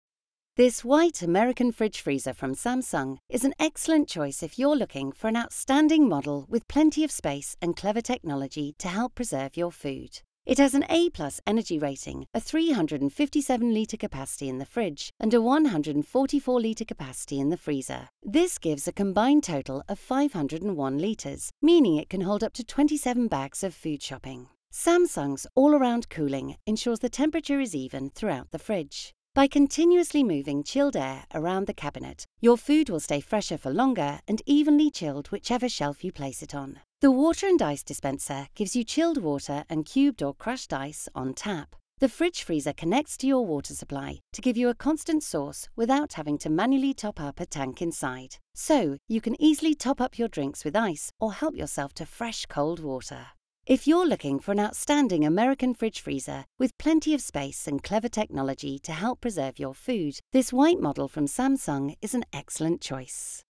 Samsung American Fridge Freezer RS50N3513WW Product Overview | ao.com
Discover all you need to know about the RS50N3513WW_WH American fridge freezer from Samsung in our short product video. All Around Cooling technology means food stays cool on every shelf, thanks to the cool air continuously circulating around the fridge. The ice and water dispenser gives you ice and chilled water on tap whenever you need it.